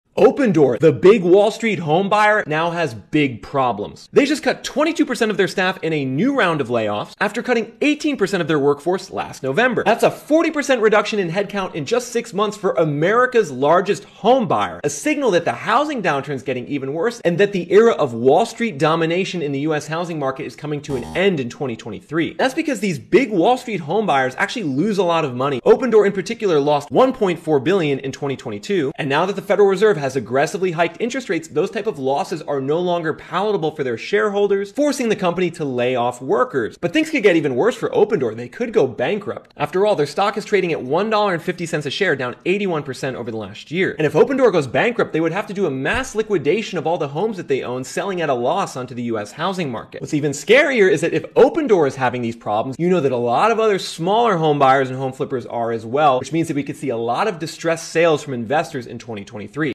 Wall Street Homebuyers doing MASS LAYOFFS (40% Cuts at Opendoor)
After Opendoor just laid off 40% of their workforce in 6 months, it's clear that even the biggest homebuyer in America is struggling in the face of the housing market downturn in 2023.

With losses of $1.4 billion in 2022 and stock prices down 81% over the last year, bankruptcy could be on the horizon for Opendoor.

And if Opendoor falls, the impact on the housing market could be catastrophic, with a fire sale of homes sold at a loss.

The era of Wall Street's iron grip on the US housing market is coming to a close...